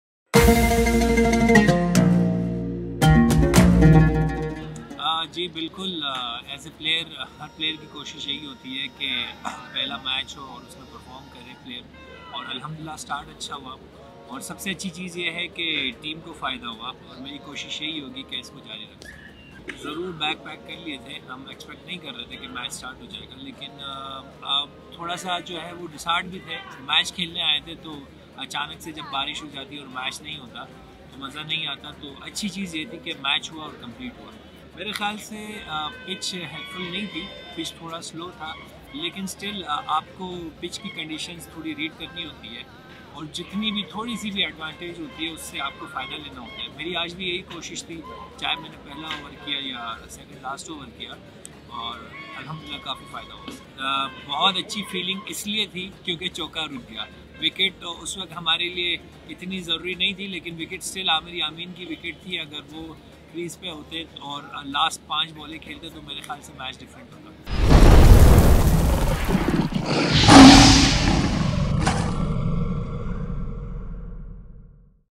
Mir Hamza | Tiger of the Match | Muzaffarabad vs Bagh | KPL 2022
Post-Match talk with the Tiger 🐯 of the day!

Mir Hamza is awarded with the Player of the Match award 🎖 for his 2 wickets for 17 runs and a superb catch.